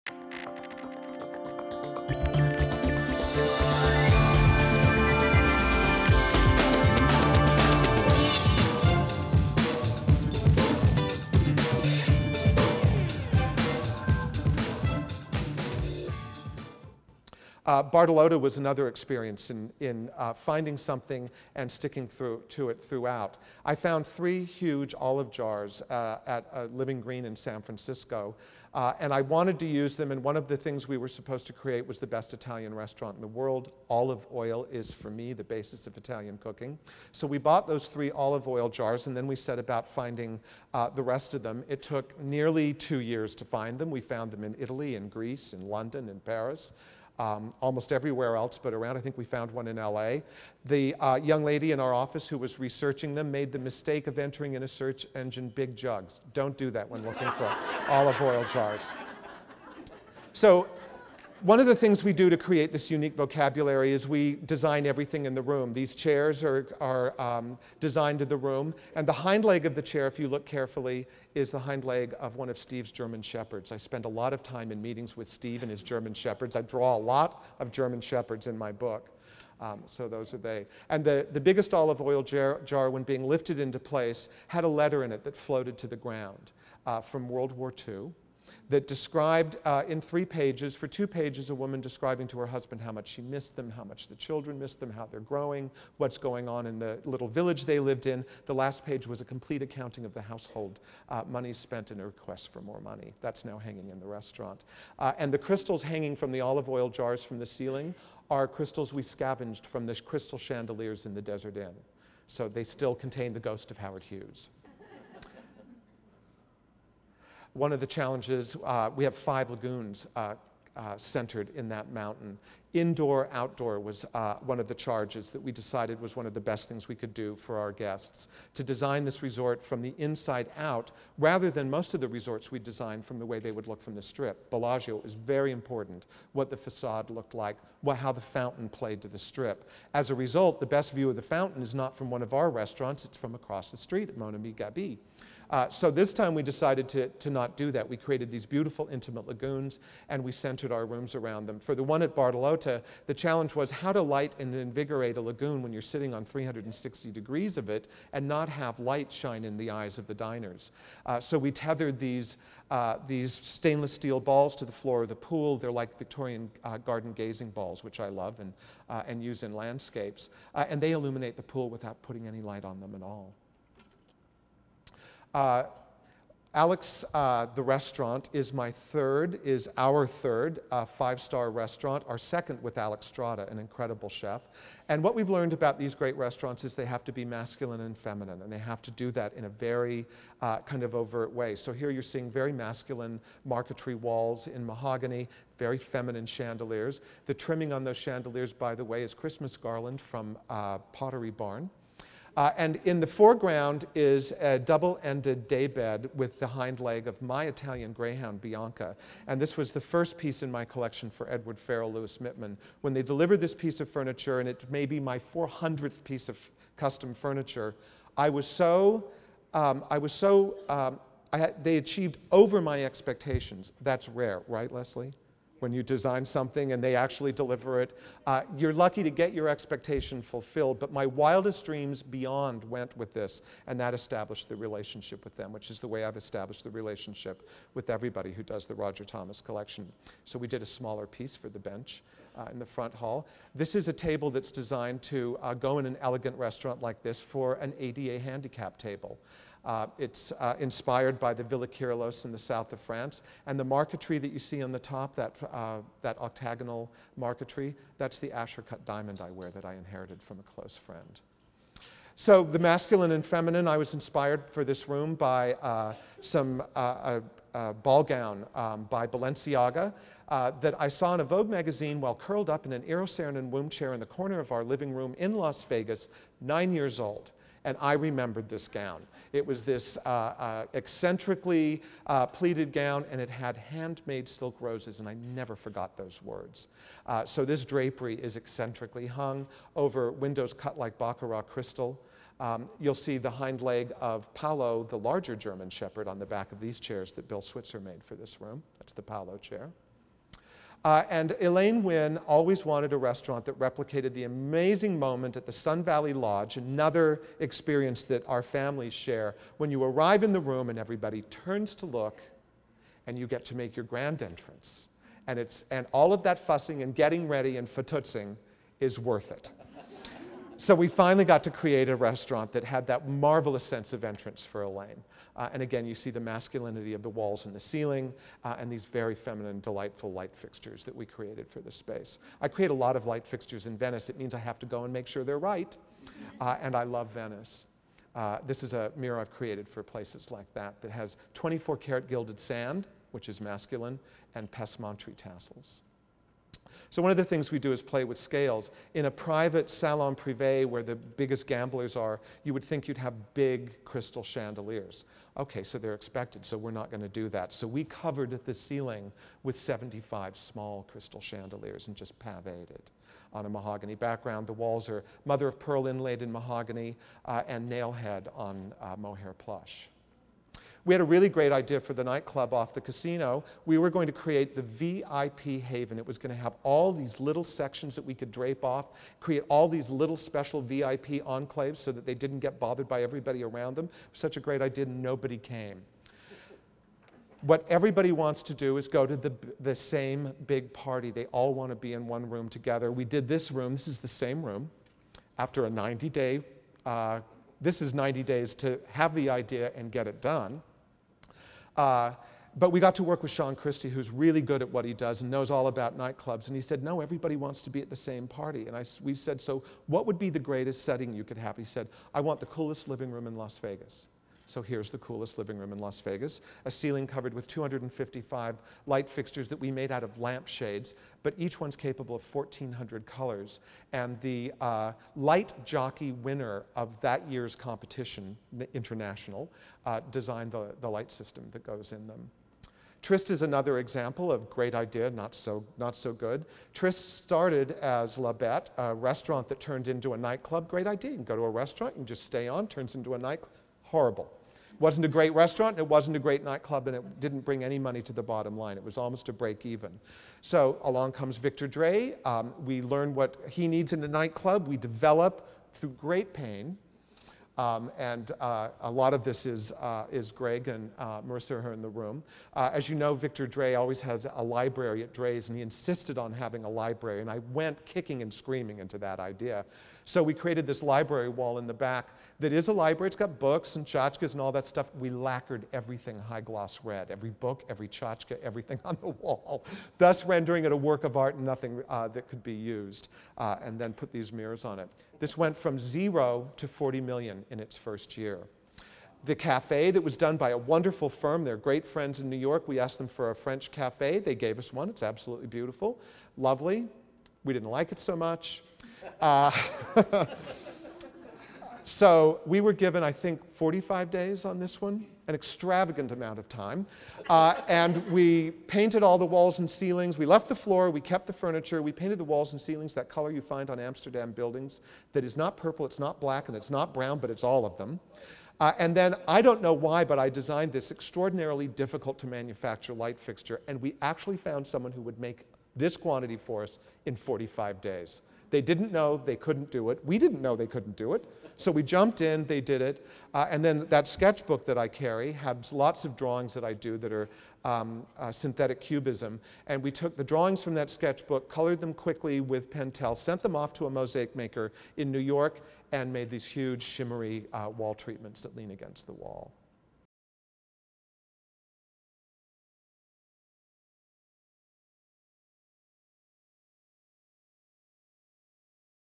Roger Thomas Design Icon Chapter 2
World Market Center Las Vegas honored Roger Thomas of Wynn Design and Development as a Design Icon during the Winter 2009 Las Vegas Market.

Thomas created the interiors of Wynn Las Vegas and most recently led the interior design of Wynn Macau, which opened to the public September 5, 2006. Encore at Wynn Las Vegas, opened on December 22, 2008, along with Wynn Diamond Macau, which will open January 2010, also bear his stamp. Thomas designed the interiors of properties including the Golden Nugget hotels in Las Vegas and Laughlin and the Mirage Resort, Treasure Island and Bellagio Resort on the Las Vegas strip.

Honors are nothing new to the noted design legend, whose career spans nearly 40 years. (He started his career as an apprentice designer while still in his teens.) Elected to the Hospitality Design Platinum Circle in 2005, Thomas received a Lifetime Achievement award from the Network of Executive Women in Hospitality.  In 2007, Thomas was listed in Architectural Digests AD 100.